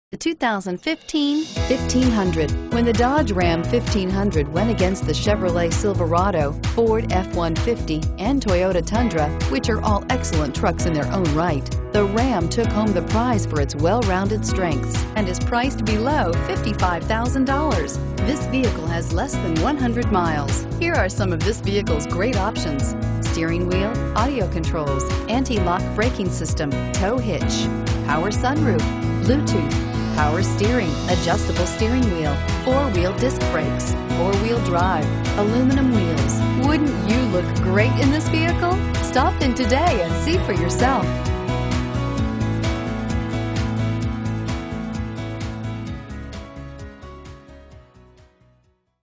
2015 Ram 1500 FS627640
2015 RAM 1500 Paramus, NJ
Stock# FS627640

Looking for the right car? Today could be your lucky day. With 21 miles, this Deep Cherry Red Crystal Pearlcoat, 2015 Ram 1500 equipped with Automatic transmission could be yours. Request more information and set up a test drive right away.